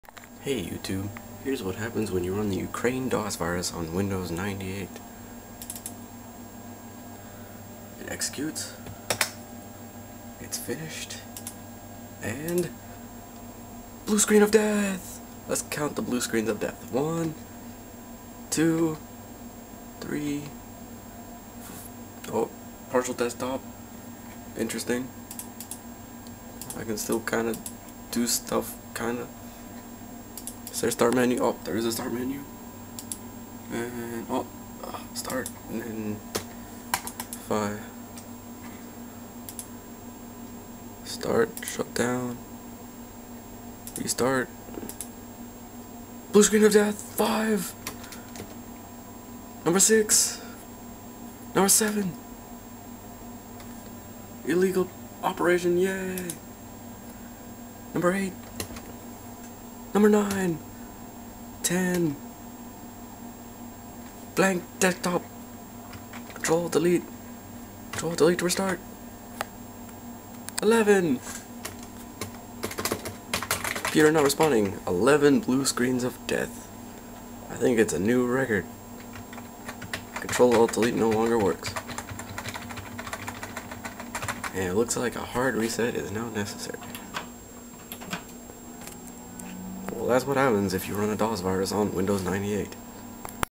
Running the Ukraine Virus on Windows 98
Here's what happens when you run the Ukraine DOS Virus on Windows 98. 11 BSOD's.

EDIT: Yeah, I know it's 10 BSOD's, and I missed number 4. You don't need to point it out for the billionth time, I don't really care, so comments are now disabled.